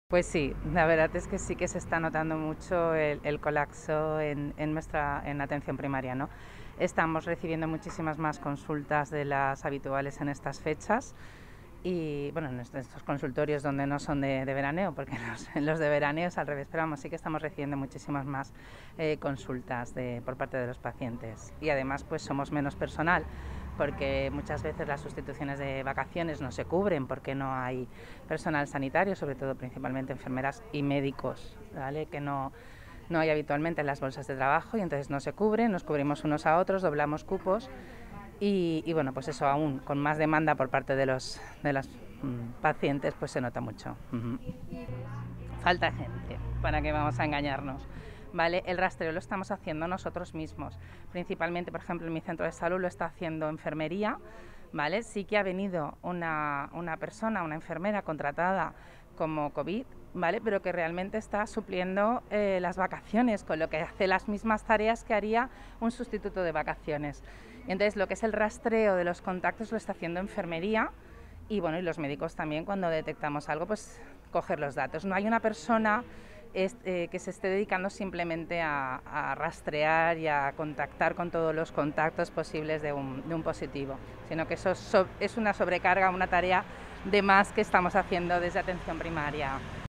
Médicos de familia denuncian un "colapso" en la atención primaria
La secretaria de la Sociedad Valenciana de Medicina Familiar y Comunitaria (SoVaMFIC) y miembro del Sindicato Médico de la Comunidad Valenciana (CESM CV), Pilar Valero, ha denunciado el "colapso" que vive la atención primaria en los hospitales y centros de salud de la Comunidad Valenciana.

La representante ha advertido de que las tareas de rastreo las llevan a cabo desde atención primaria, lo que supone una "sobrecarga" para el personal. "No hay una persona que se dedique simplemente a rastrear y contactar con todos los contactos posibles de un positivo", ha explicado en declaraciones a Europa Press Televisión.